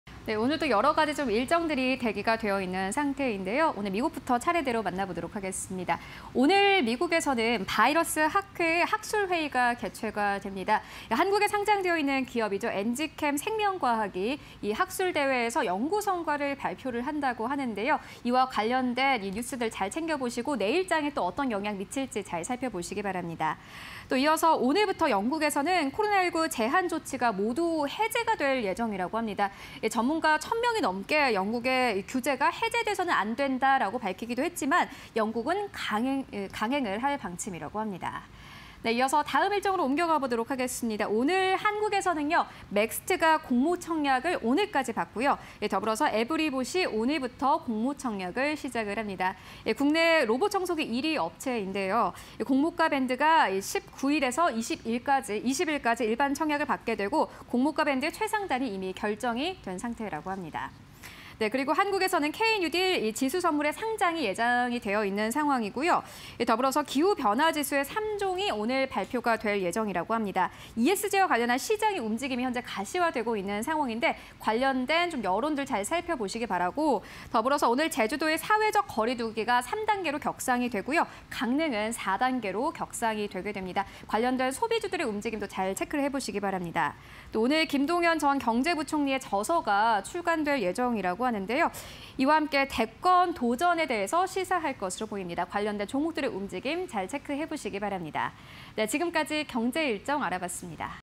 美 바이러스학회 개최… 엔지켐생명과학 연구 성과 발표_오늘장 글로벌 일정 (20210719)
미국 : 바이러스학회 학술회의 개최
미국 : 7월 주택시장지수
미국 : IBM 실적 발표
영국 : 코로나19 제한 조치 해제 예정

한국 : 맥스트 공모청약
한국 : 에브리봇 공모청약
한국 : K-뉴딜 지수선물 상장 예정
한국 : 민주노총 온라인 대의원대회

한국 : 제주도, 사회적 거리두기 3단계 격상
한국 : 기후변화지수 3종 발표 예정
한국 : 김동연 전 경제부총리 저서 출간